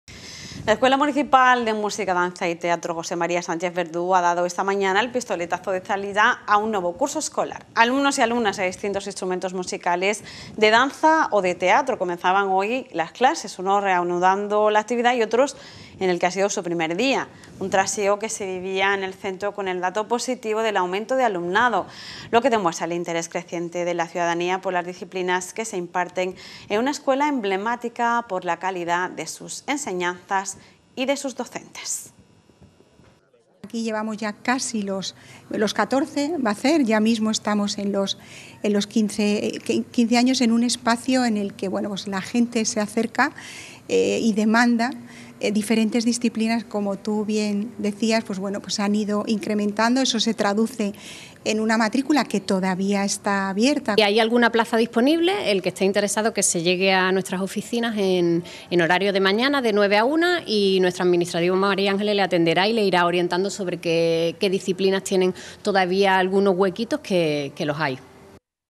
Arranca el nuevo curso en la Escuela Municipal de Música, Danza y Teatro José María Sánchez Verdú
La Escuela Municipal de Música, Danza y Teatro José María Sánchez Verdú ha dado esta mañana el pistoletazo de salida al nuevo curso escolar. Alumnos y alumnas de distintos instrumentos musicales, de danza o de teatro comenzaban hoy las clases, unos reanudando la actividad y otros en su primer día. Un trasiego que se vivía en el centro con el dato positivo del aumento del alumnado, lo que demuestra el interés creciente de la ciudadanía por las disciplinas que se imparten en una escuela emblemática por la calidad de sus enseñanzas y de sus docentes.